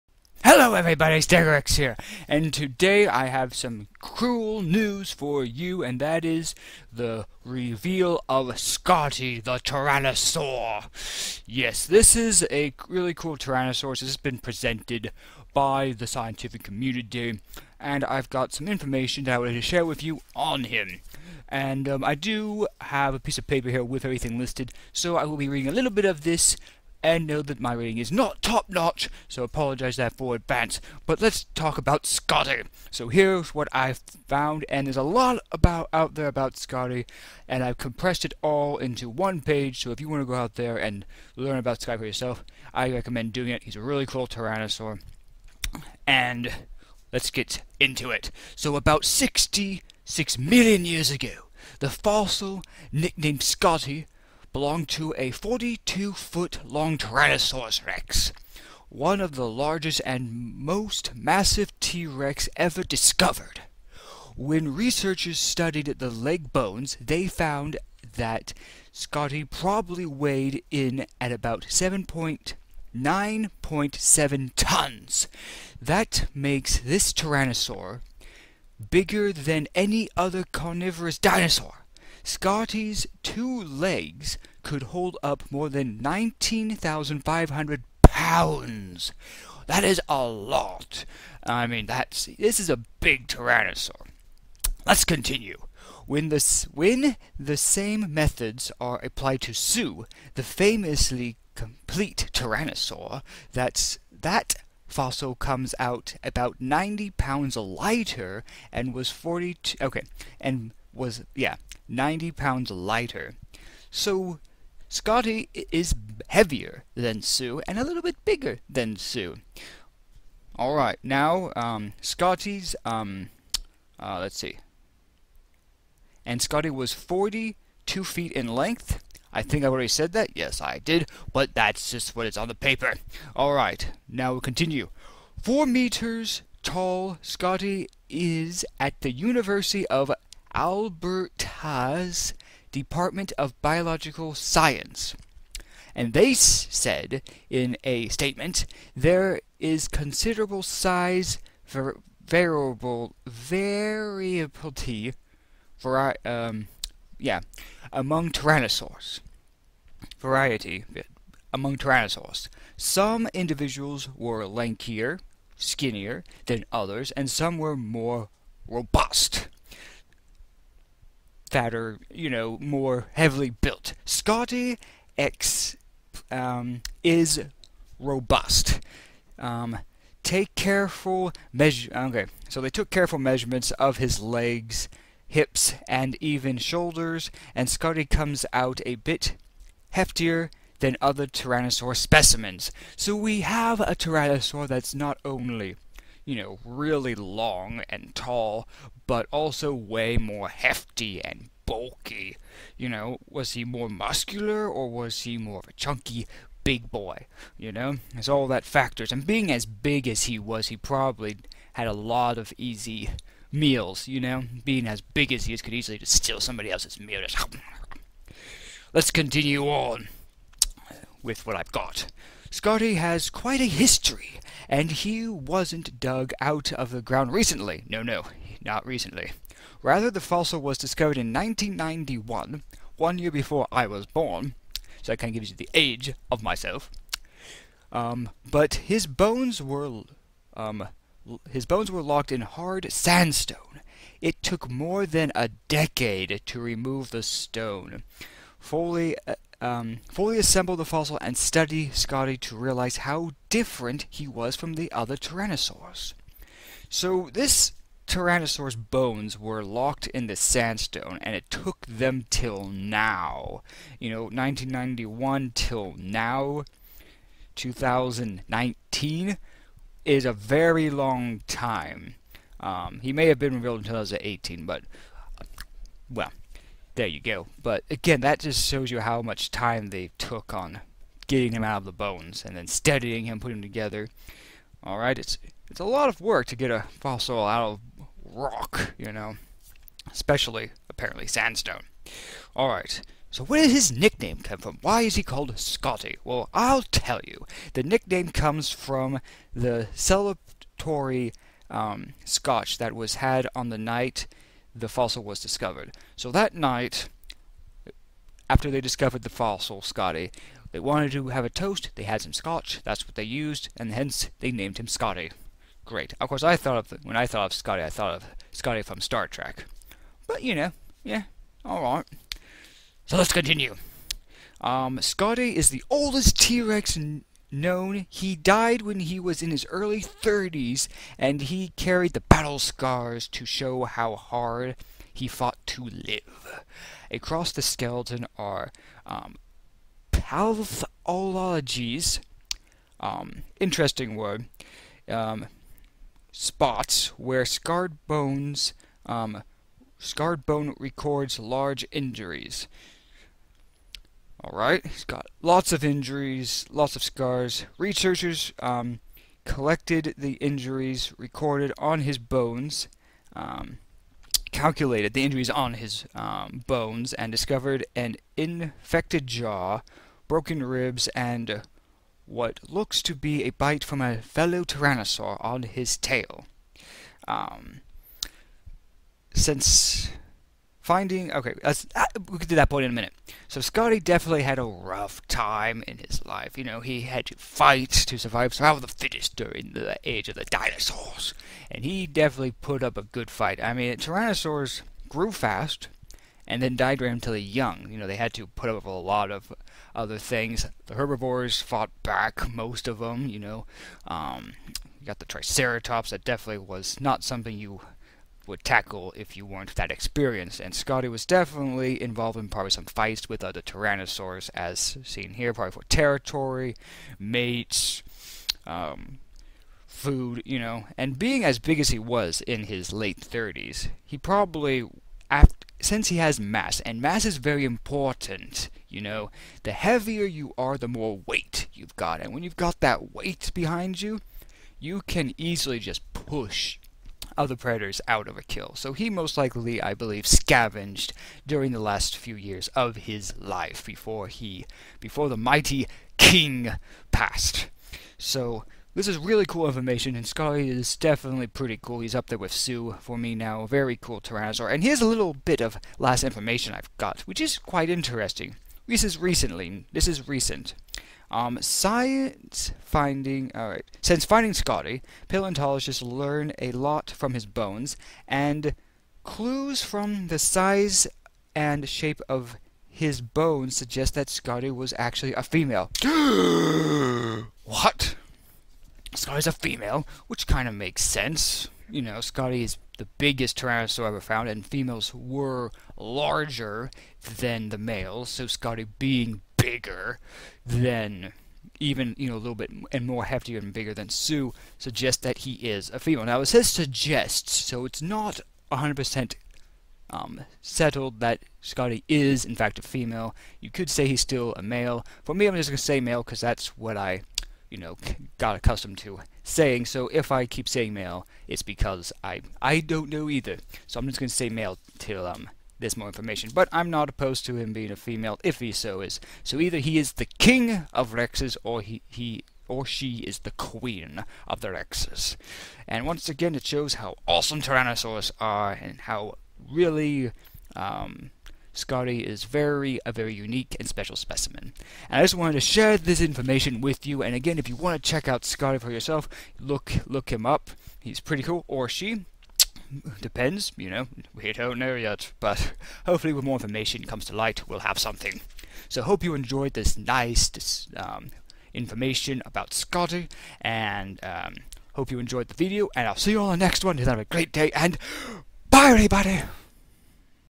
Meet Scotty, the biggest T Rex Ever Found
In this video I talk about Scotty the biggest and lagest T rex that has been found so far.